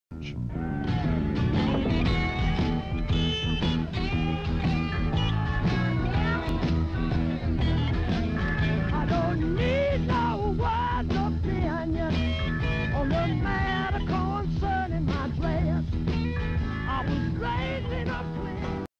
1960s, 1970s UK Music Festival | Kinolibrary
1960s, 1970s UK Music Festival from the Kinolibrary Archive Film Collections. To order the clip clean and high res or to find out more Clip ref VA3.

People at festival carrying rock, men taking photograph. Crowds.